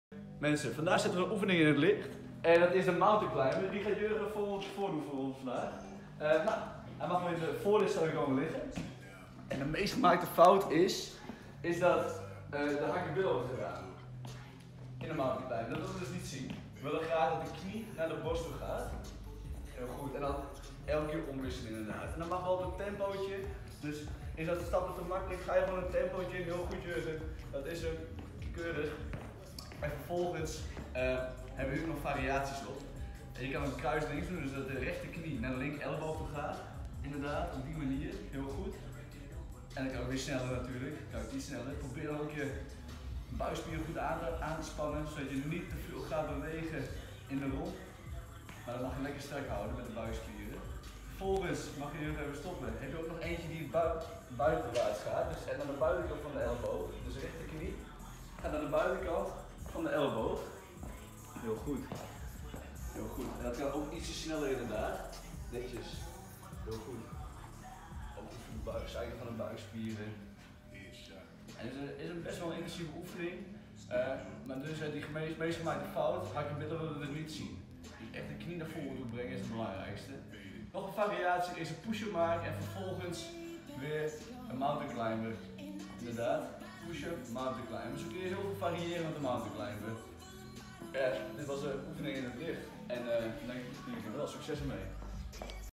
05 | De Mountain Climber
De Mountain Climber 🏔
Goed voor niet alleen de buikspieren!
Hier een gedetailleerde uitleg van Robin.